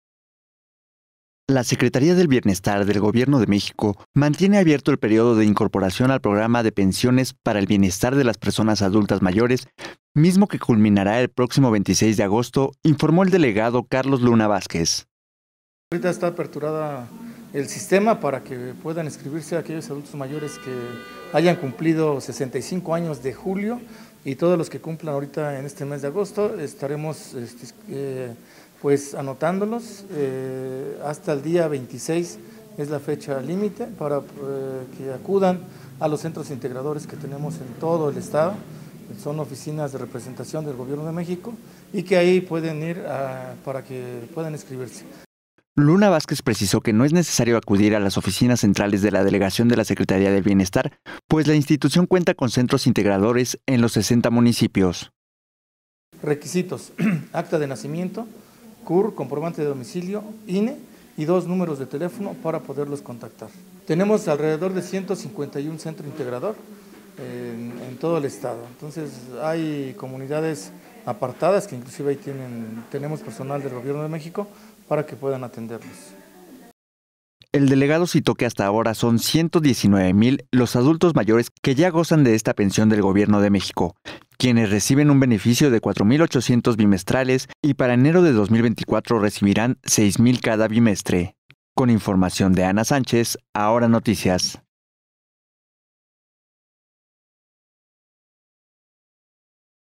Continuan abiertas inscripción programa de Pensión para el Bienestar de las Personas Adultas Mayores
La Secretaría del Bienestar del Gobierno de México, mantiene abierto el período de incorporación al programa de Pensión para el Bienestar de las Personas Adultas Mayores, mismo que culminará el próximo 26 de agosto